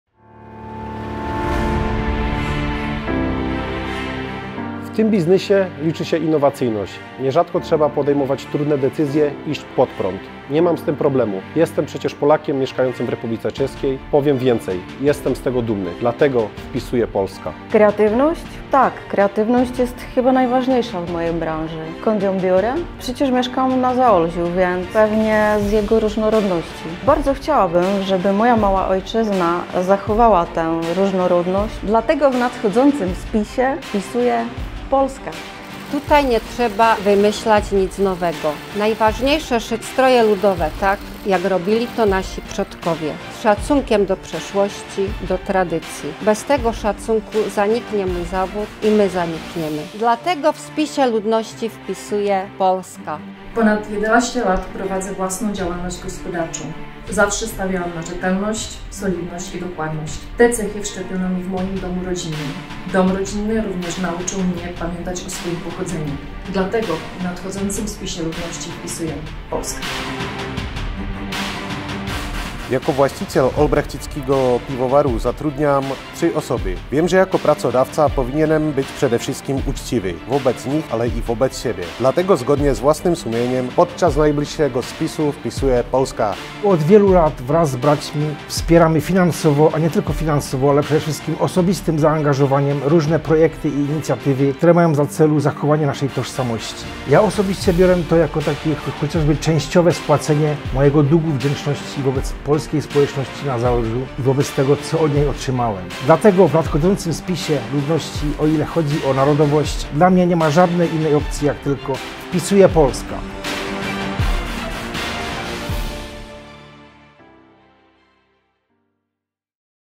Polskość niejedno ma imię - Wpisuję: polská
Polacy w Republice Czeskiej imponują zaradnością, kreatywnością i pracowitością. Film, wyprodukowany przez Centrum Polskie Kongresu Polaków w Republice Czeskiej, poświęcony jest właśnie przedsiębiorcom.

Jeżeli jesteś ciekaw ich historii, koniecznie obejrzyj ten spot!

Wystąpili: Marek Glac - Lovelec, Beata Haltof Malování Třinec, Renata Staś RS Folk, Dorota Kukuczka Jurzyca - nowaspolka.eu, Łukasz Niemiec - Albrechtický pivovar, Waldemar Wałach - Aternus.

Scitani2021 - Spis ludności 2021 jeżechpolok - Wpisuję: polská